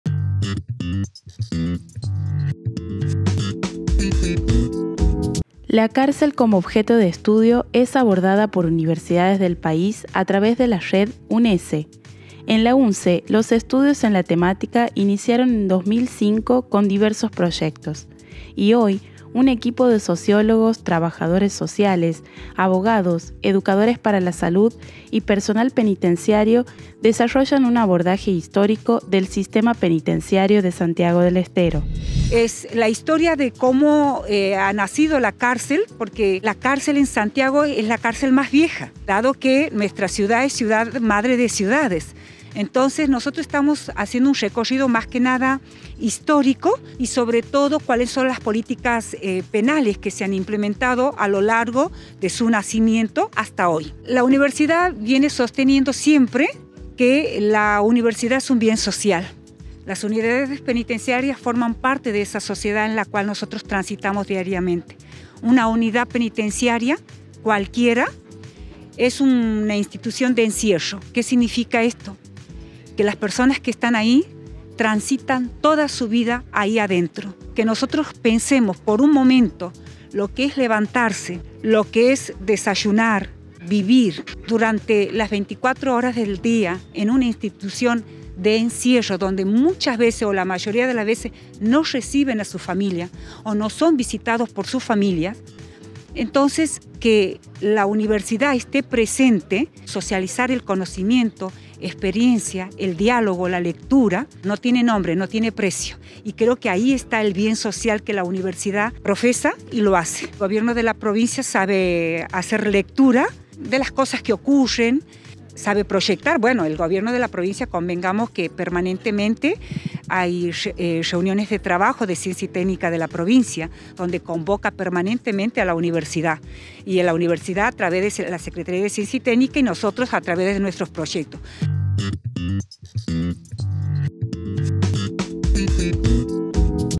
Estudios de la UNSE recuperan la historia de las cárceles en la provincia
🔹 En la UNSE los estudios sobre las cárceles iniciaron en 2005 con diversos proyectos. El sistema penitenciario como objeto de estudio es abordado por universidades del país a través de la Red UNECE. Hoy un equipo de sociólogos, trabajadores sociales, abogados, educadores para la salud y personal penitenciario, desarrollan un abordaje histórico de la Santiago del Estero como sede de la primera cárcel del país.